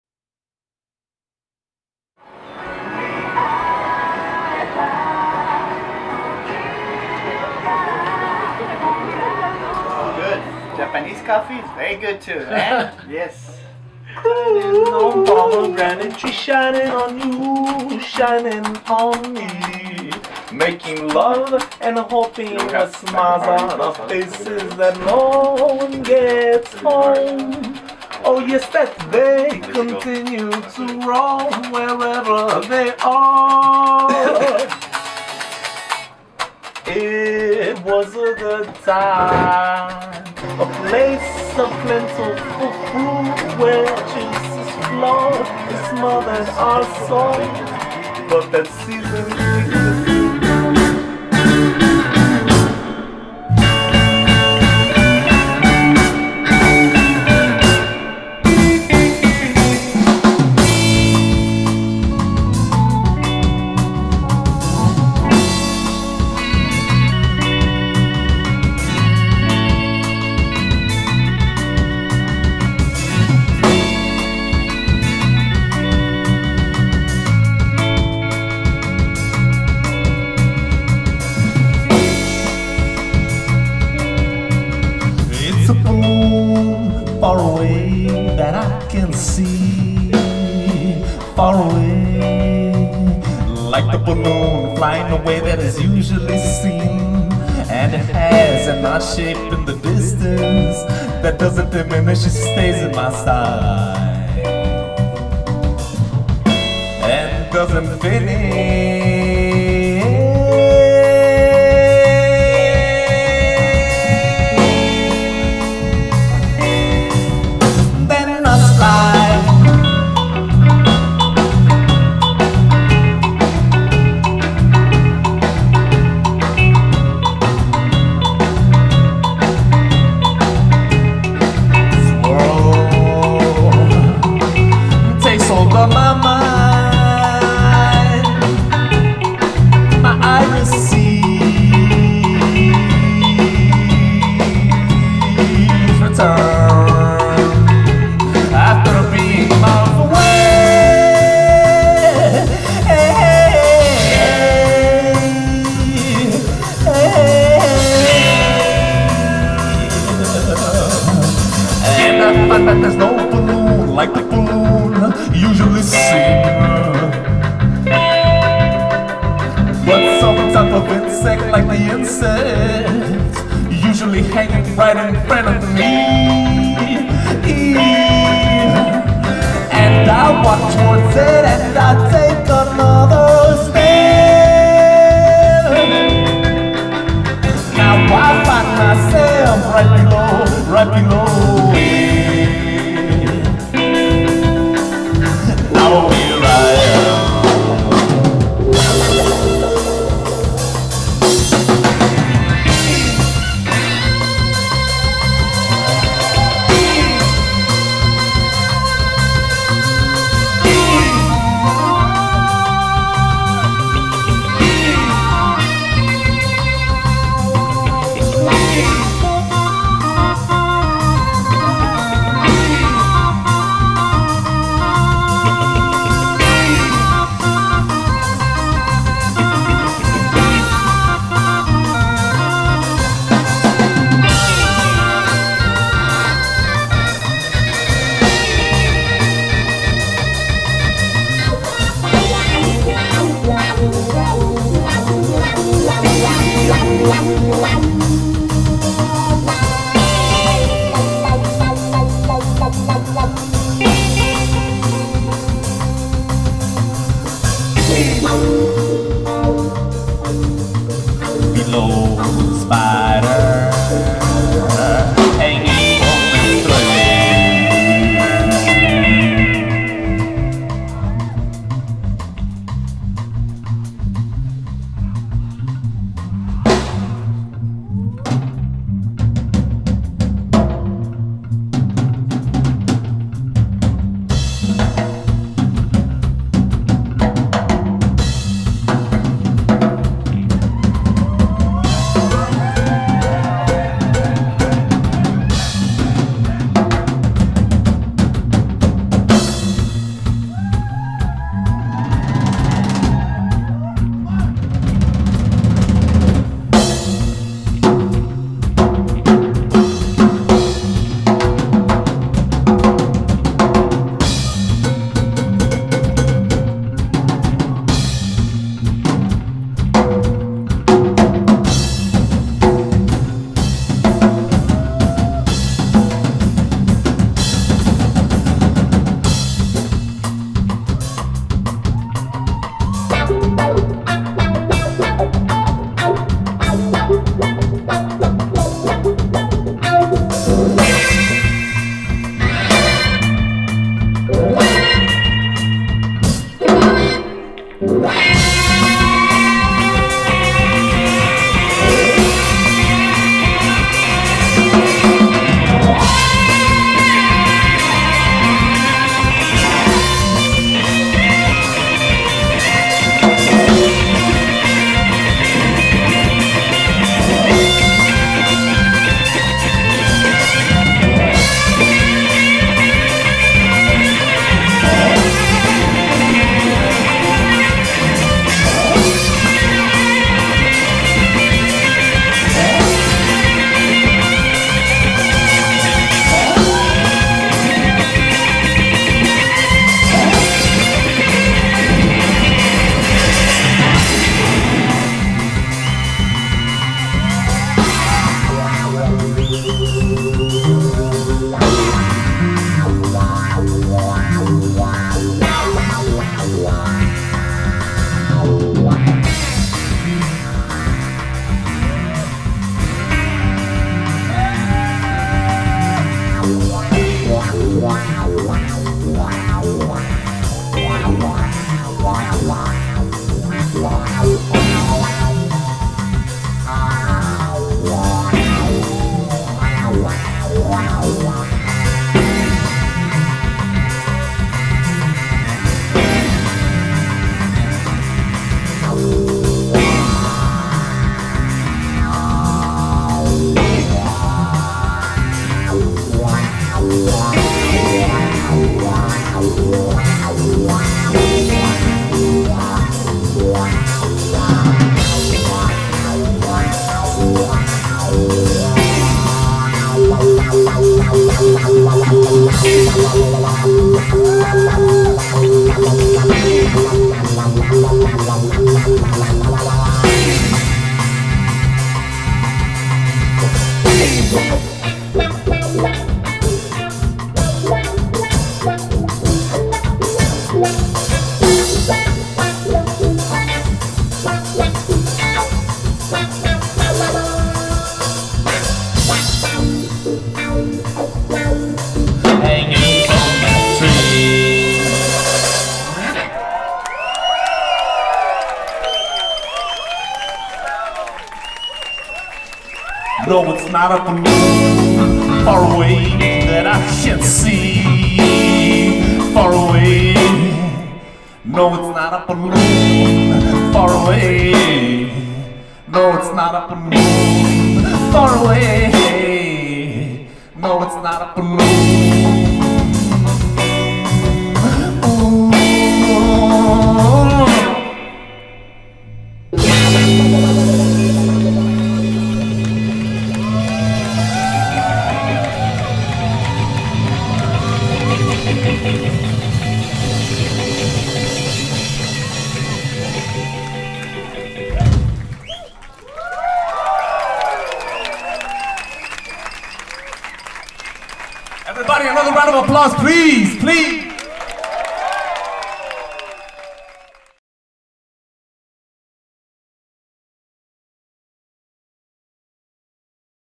CHICANO BATMAN LIVE IN JAPAN
@Tsuki Miru Kimi Omou, Minato-ku, Tokyo on Nov.11, 2013.
All Chicano Batmans' CDs are available in Japan via BARRIO GOLD RECORDS/MUSIC CAMP, Inc.